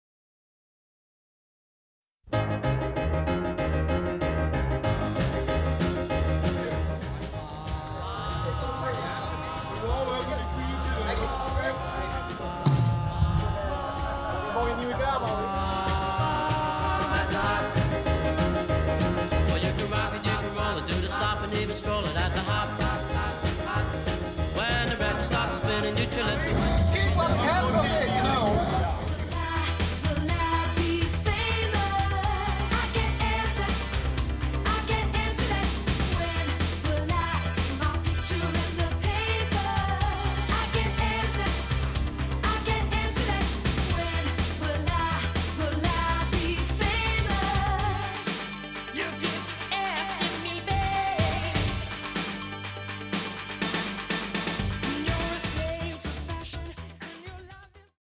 MA-Run 2011.mpg
Verslag van een jaarlijkse gehandicaptenrun met zijspannen te Venlo. Uiteraard met bijbehorende feestavonden!